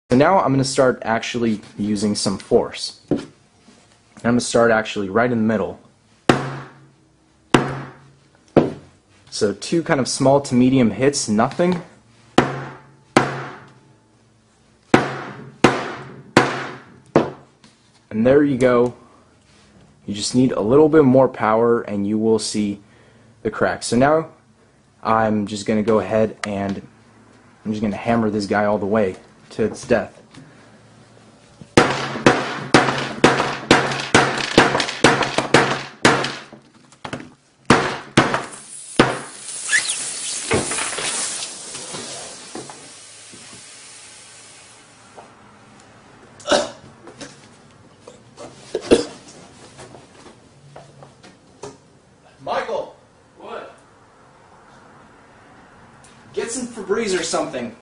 Samsung Galaxy S5 Hammer Test Fail - Wait for it
This Samsung phone wasn't going down without a fight. Did Michael every come through with that Febreze?

To anyone curious as to whether or not this means the battery in the Galaxy S5 is prone to dangerous malfunction... remember, this one took a direct hit from a fucking sledge hammer.

Pretty sure you're going to piss off any battery you do that too.